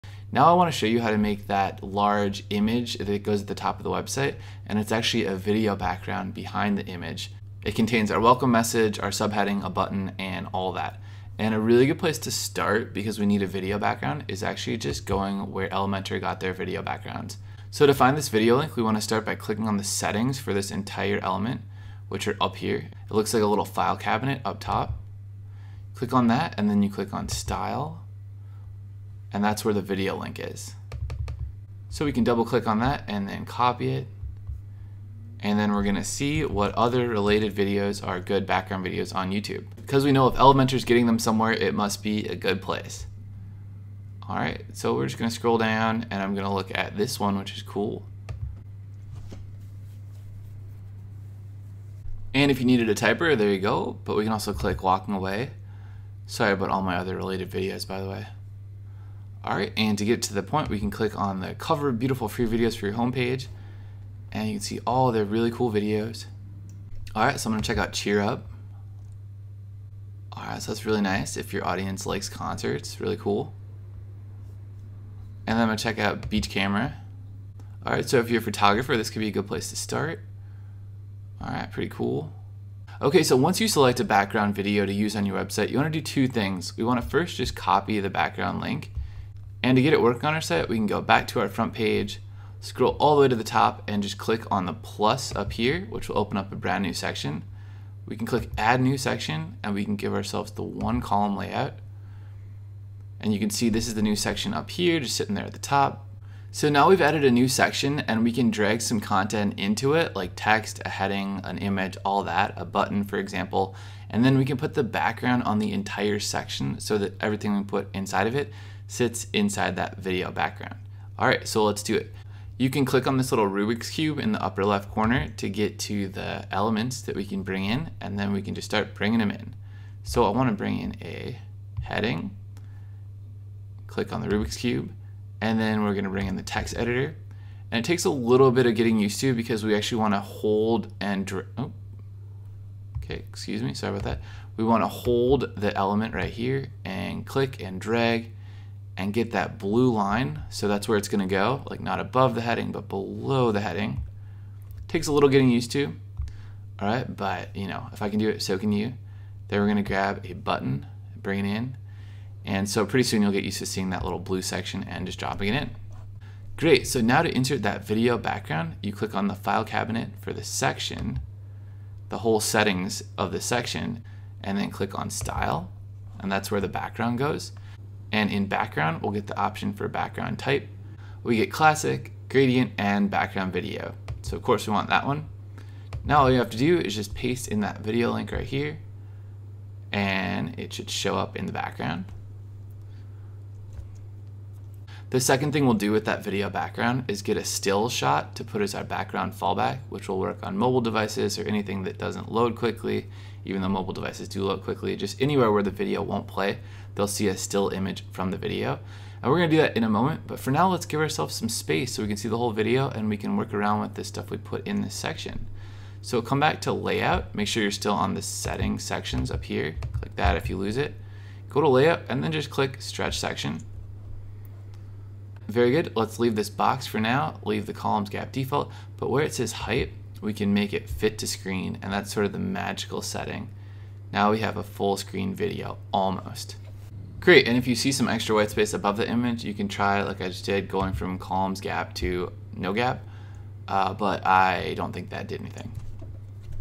Create Video Background in Elementor - #13
It's easy to create a video background in Elementor. Just go to column settings, style and choose the background type for video, then insert your video. We also see how to create a full width video background.

Whatever you want to create, I want to help, and am always here (on YouTube, FB, Twitter, and Email) for you.